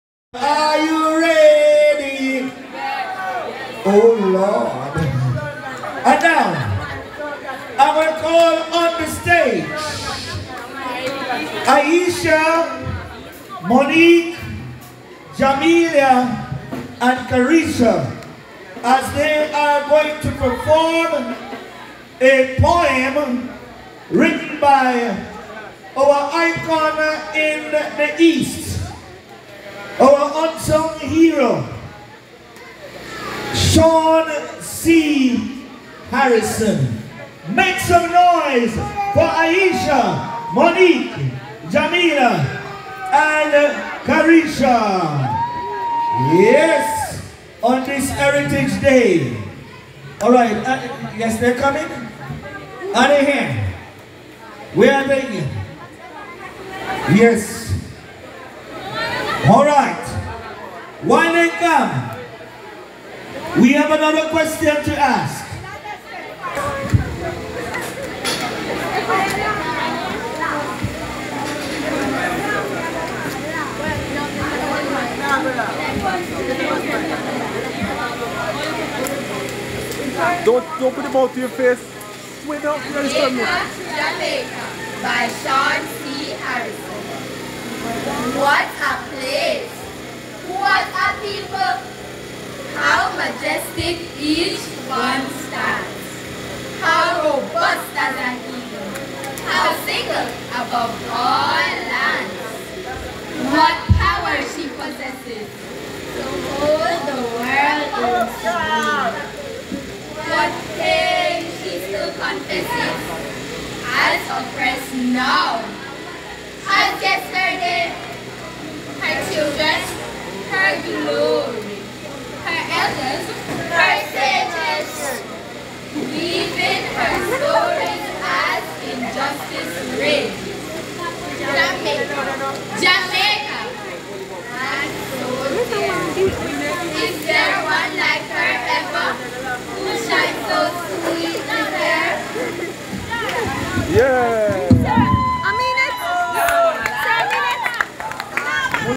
Jamaica! Jamaica! Recital By Paul Bogle High School Students
Students of the Paul Bogle High School recite Sean C. Harrison's patriotic poem entitled " Jamaica! Jamaica! at Heritage Week 2022.